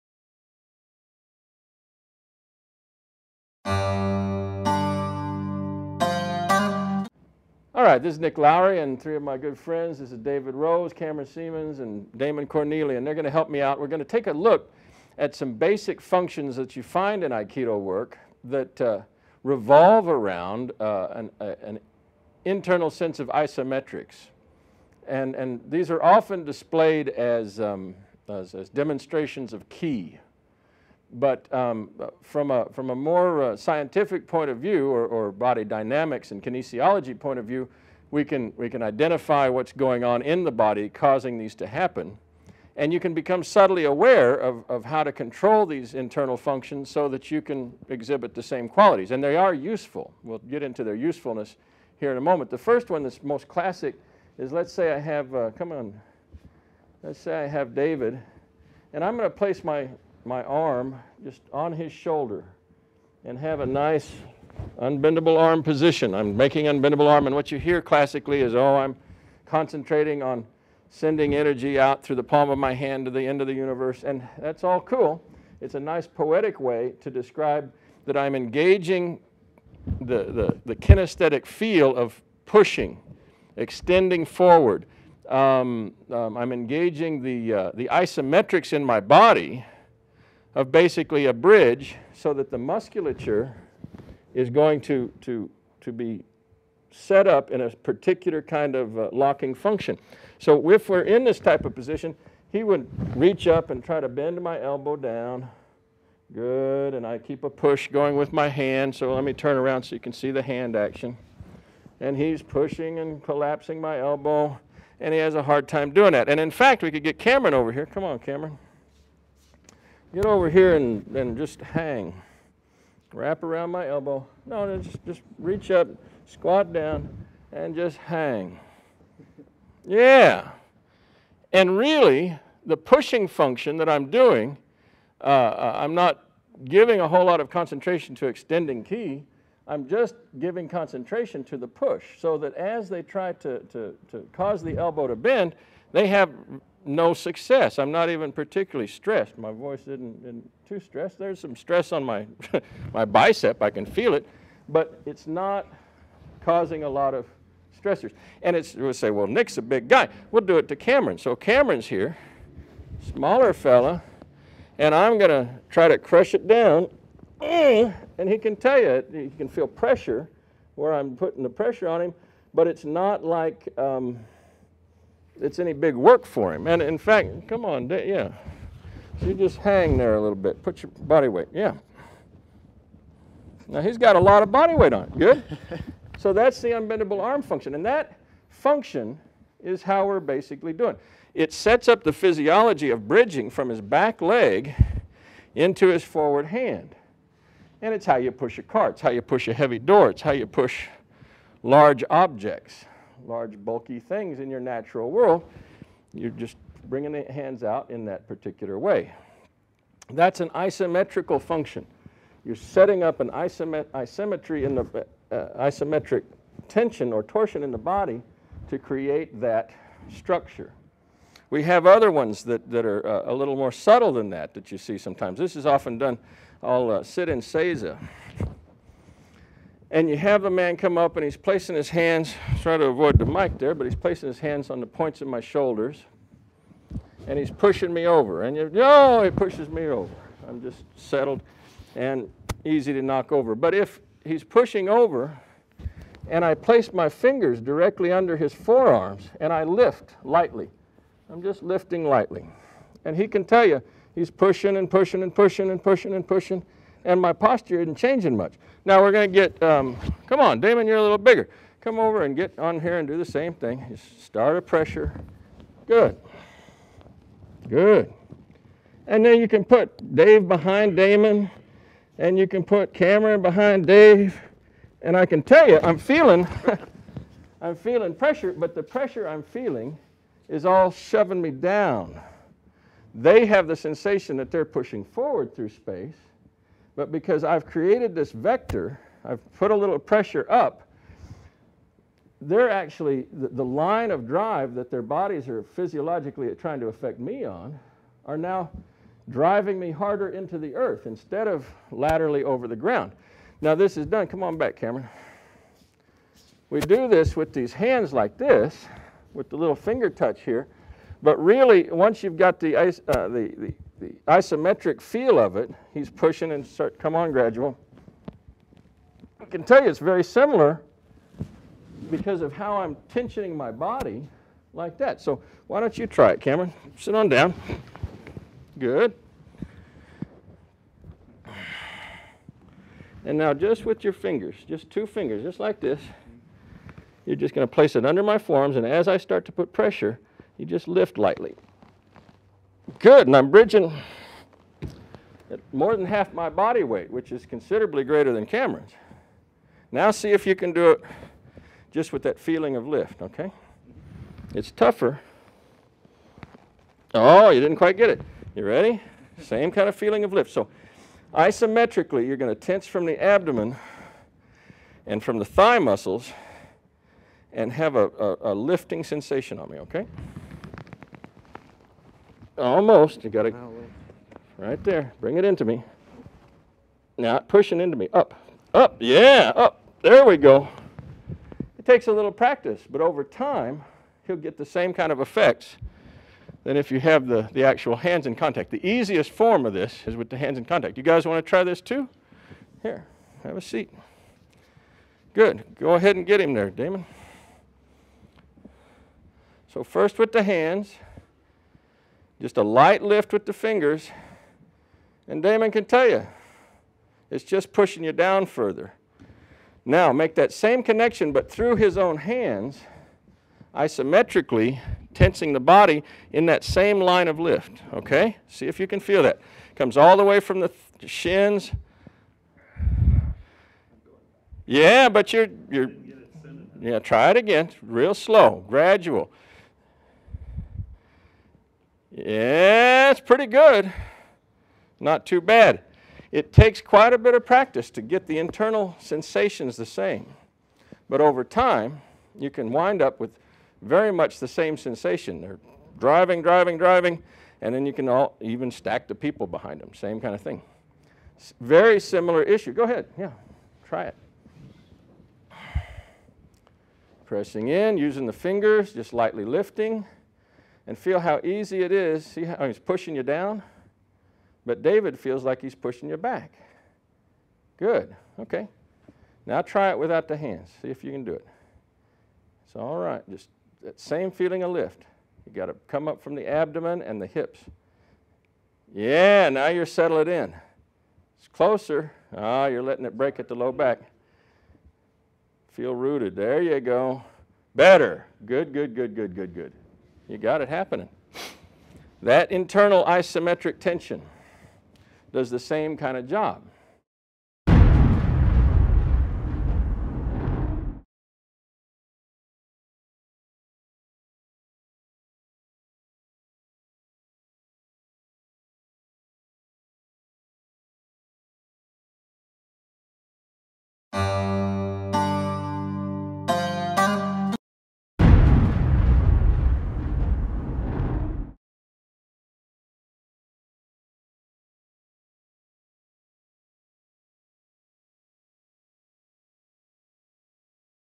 Aikido Principles: Ki - Isometrics and Vectors, part 1 of 4
Nick and his students David, Cameron, Damon explore demonstrations of ki - the delivery of energy - in aikido based on body movement. Energy in aikido is often discussed in terms of movement and action, but not explained in terms of what is actually happening in a person's body to deliver that energy. Nick takes a scientific approach, exploring what is happening with a person's body mechanics when delivering energy

0:00 Introduction
0:52 Isometrics of Unbendable Arm
3:40 Lifting/pushing vector from seiza position
6:00 Tensing the abdomen to lift